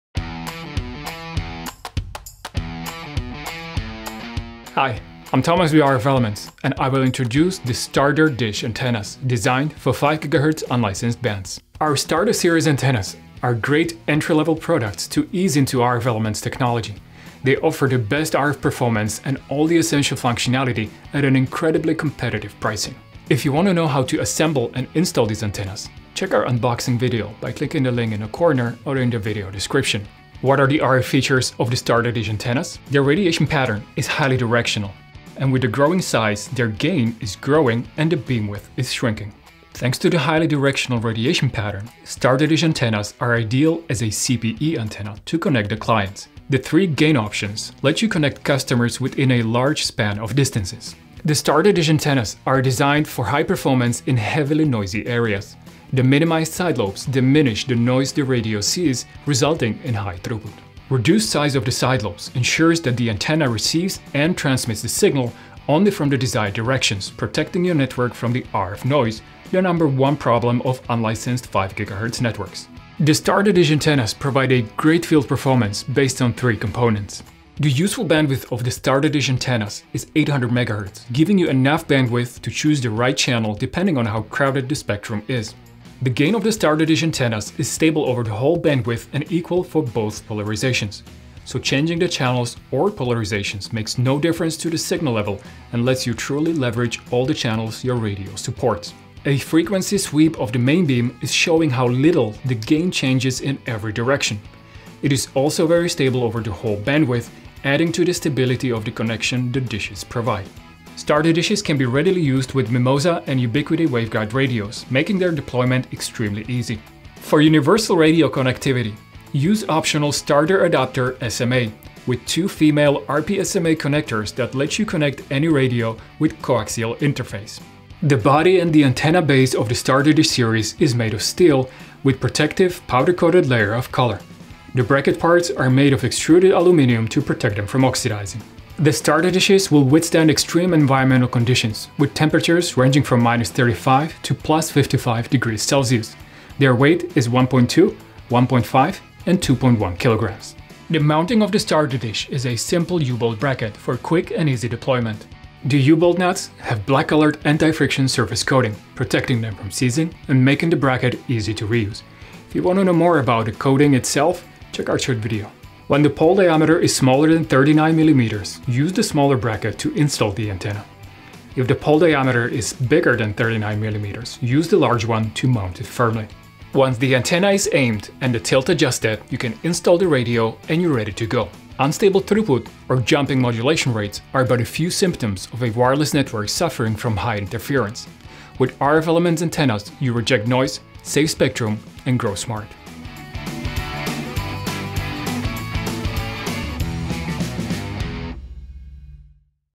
Product Introduction: StarterDish Antennas
RF elements StarterDish antennas are great entry level products to ease into RF elements technology. Starter antennas deliver the best RF performance and all the essential functionality for a very competitive price. They are easy to use with the radio platform you already have. Starter antennas support SMA coaxial interface and also third party waveguide connectors.

The radiation pattern of a dish antenna is highly directional. With increasing size of the StarterDish the main lobe beam width gets narrower and the gain grows. Thanks to the highly directional radiation pattern, StarterDish antennas are ideal as a CPE antenna to connect the clients - the three gain options  of 21, 24, and 27 dBi let you connect customers within a large span of distances.

The StarterDish antennas are designed for reliable performance in high noise areas - the minimized SLs diminish the noise the radio sees resulting in high throughput. The minimized SLs and narrow beam of StarterDishes makes them perfect antennas for dense deployments as client stations. Reduced size of SL ensures that the antenna receives and transmits the signal only from the desired directions, protecting your network from RF noise, the no. 1 problem of unlicensed 5GHz networks.

The StarterDish antennas provide a great field performance based on three components:

1. The useful bandwidth of the StarterDish series spans 800 MHz in the 5 GHz frequency range, giving you enough bandwidth to choose the right channel depending on how crowded the spectrum is.

2. The gain of the StarterDish antennas is stable over the whole bandwidth and equal for both H and V polarizations, so changing the channels or polarization makes no difference to the signal level and lets you truly leverage all the channels your radio supports.

3. Frequency sweep of the main beam is showing how little gain changes in every direction. It is also very stable over the whole bandwidth, adding to the stability of the connection the dish provides.

Radio connectivity
StarterDishes come with the option to readily use Mimosa UI waveguide radios which makes the deployment of StarterDishes very easy. For universal radio connectivity, you can get optional StarterAdapter SMA, with two female RP-SMA connectors that lets you connect any radio with coaxial interface.

RF noise, unstable throughput, or jumping modulation rates are but a few symptoms that a wireless network suffers from high interference - with RF elements antennas, you can reject noise, save spectrum, and grow smart.

Mechanical & assembly part
The body and the antenna base of the StarterDish series is made of steel with protective powder coated layer of color. The bracket parts are made of extruded aluminium to protect it from oxidizing. The mounting of the AS is an industry standard U-bolt bracket for quick and easy deployment. The U-bolt nuts have black colored anti-friction surface coating protecting them from seizing extending the longevity of t he bracket.

Once the antenna is aimed and the tilt adjusted, you can install either waveguide-based radios with the adapter included in the package, or any other radio using a pair of female RP-SMA connectors with the optional adapter, and you’re ready to go.

0:00 Intro
0:30 RF features
1:17 Three performance components
1:50 Radio connectivity options
2:07 Antenna build
2:30 Mounting